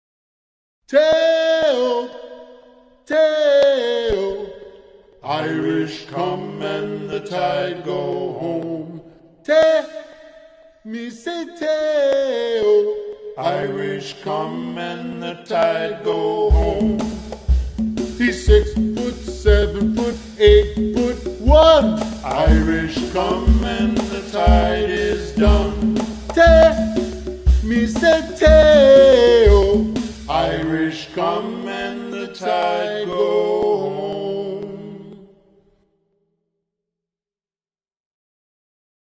Te'o; captain of The Fighting Irish; beloved by Irish fans everywhere
The Fighting Irish of Notre Dame led by Captain Manti Te'o All American from Hawaii. Is he a big enough factor for ND to overcome the nearly 10 points favoring The Crimson Tide in The BCS Championship Game? We think so!
"He's six foot, seven foot, eight foot one", in our hearts. Go Irish beat Bama.